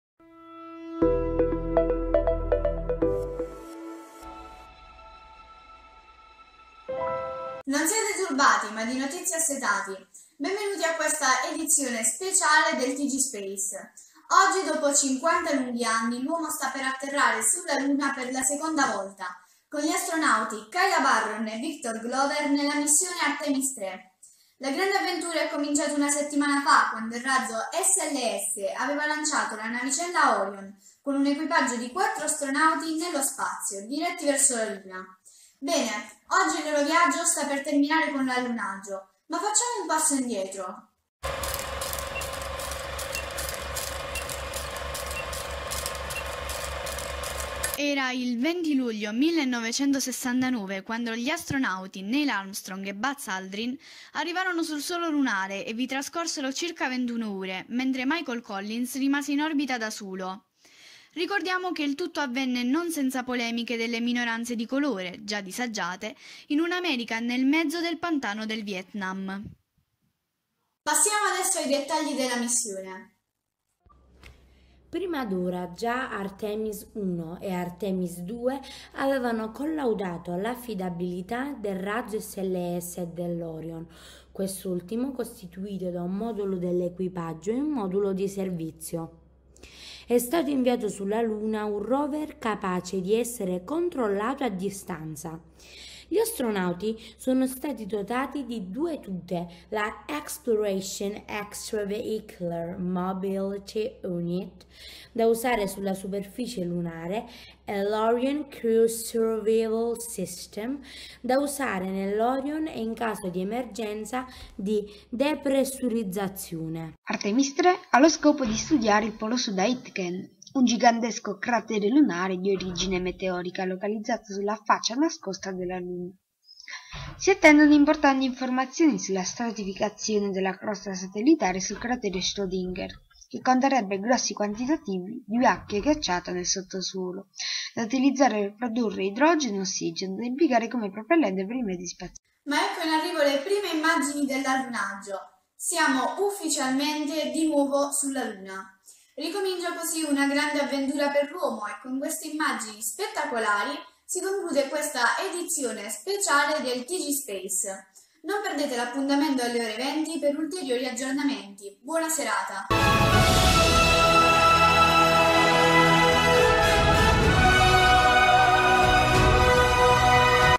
Concorso "Space dream" - Classe 2A Tg Space
Menzione speciale, categoria TG per "la cura dei contenuti e il focus su Artemis"